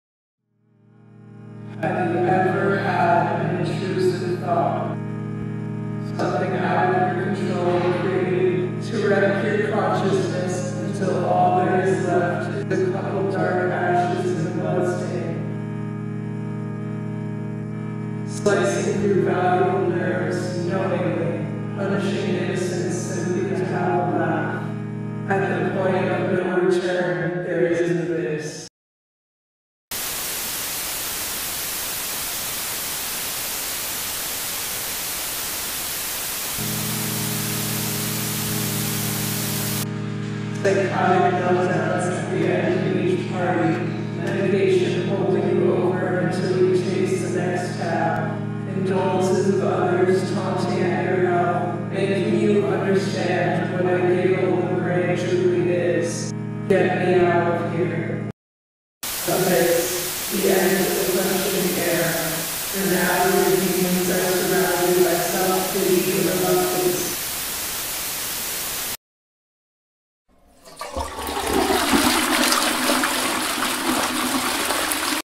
ABYSS: A Short Film
a film about falling into depression and using substances to try to heal yourself.